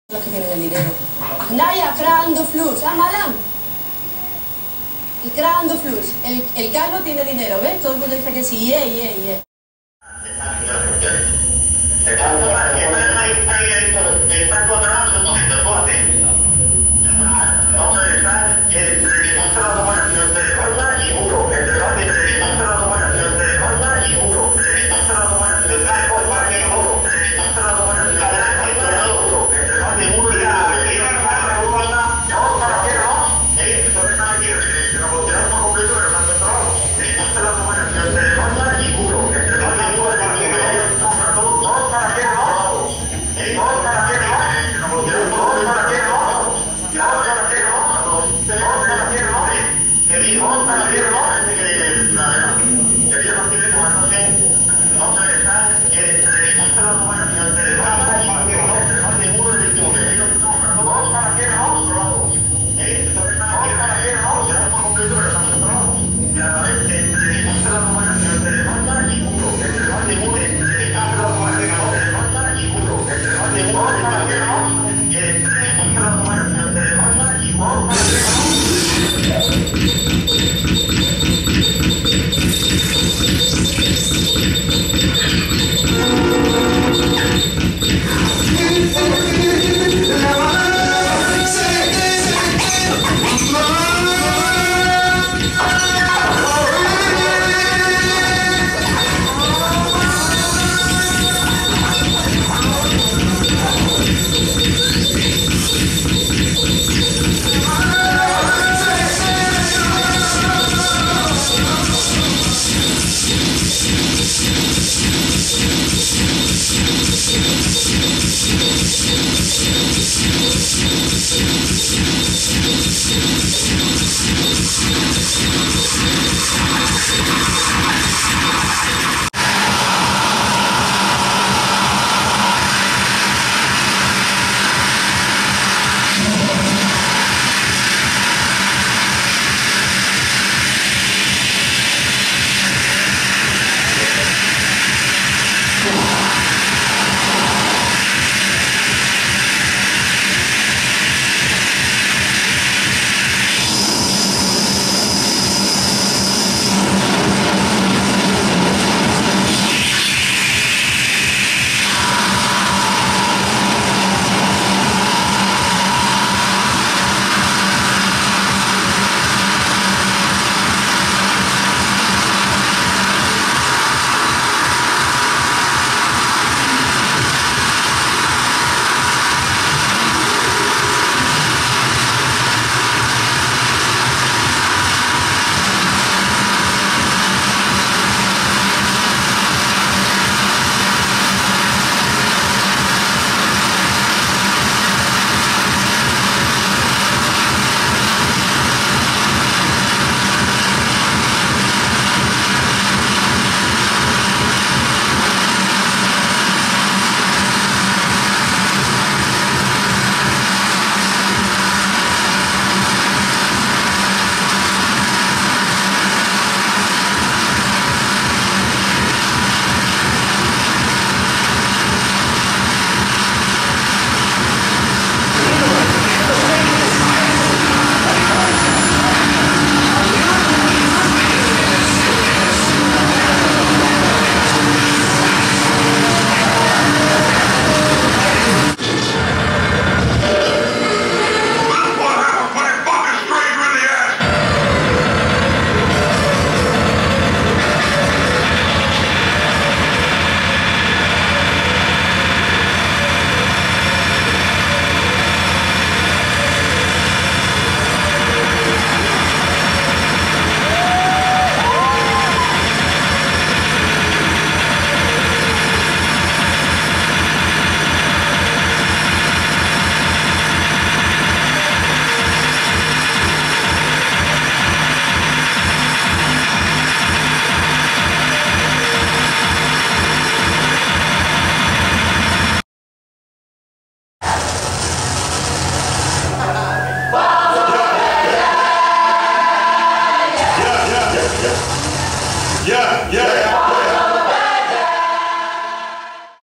Los Voluble en el CSOA La Huelga (2010)
Sevilla. En las Jornadas Cultura LIbre (2010)